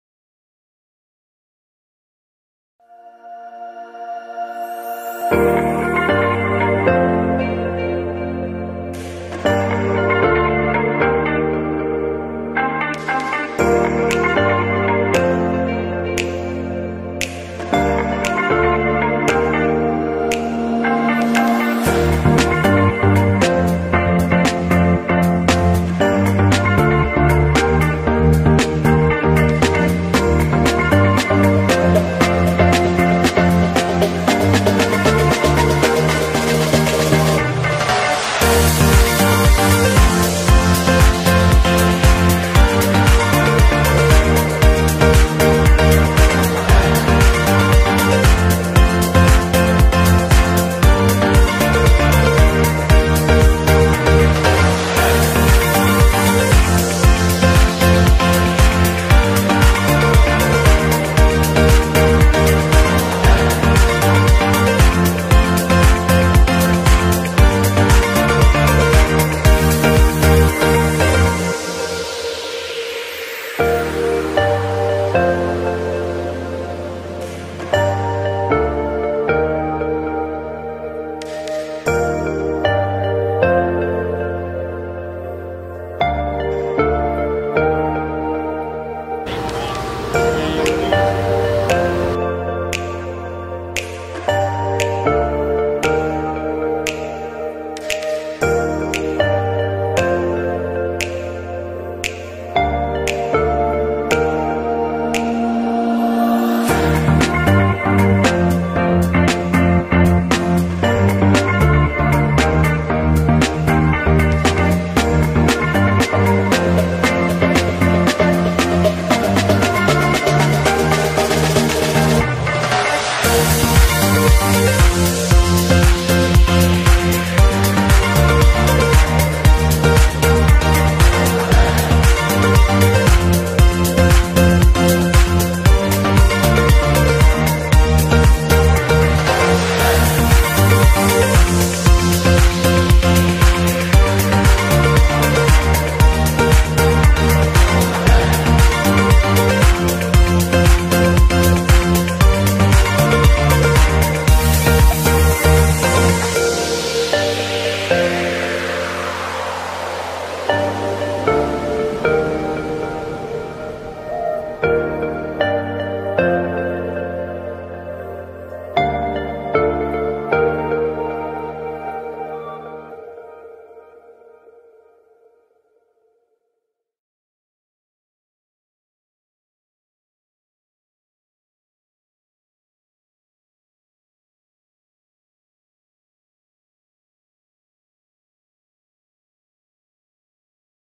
MISS Field Trip 1.0 (2019)
MINDRICH International Secondary School Field Trip 1.0
Venues: Museum & Art Gallery Bank Negara Malaysia & Skyrides Putrajaya